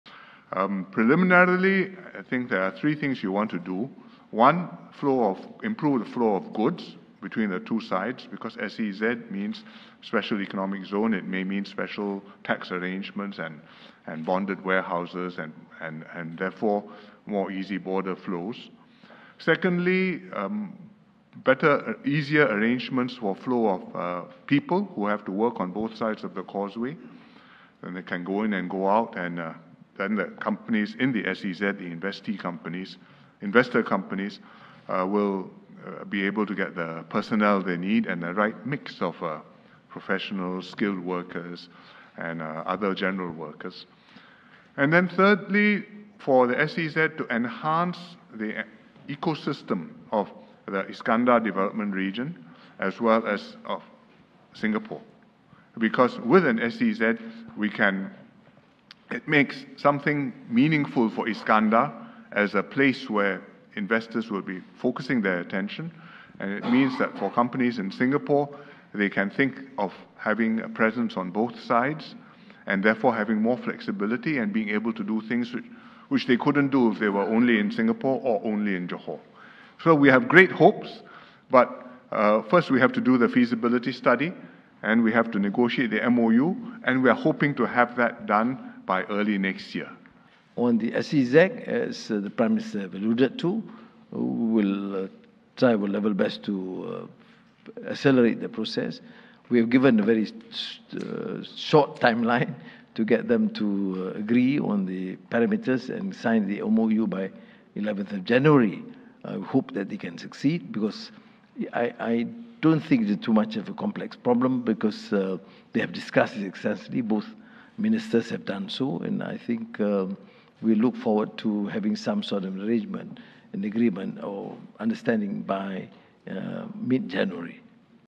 Singapore-Johor special economic zone deal to be signed in Jan 2024
Singapore and Malaysia aim to sign an agreement for a proposed Special Economic Zone (SEZ) between Johor and Singapore by mid-January next year. Prime Minister Lee Hsien Loong and Malaysian Prime Minister Anwar Ibrahim spoke about this at a press conference at the Istana on Monday (Oct 30), following the 10th Singapore-Malaysia Leaders' Retreat.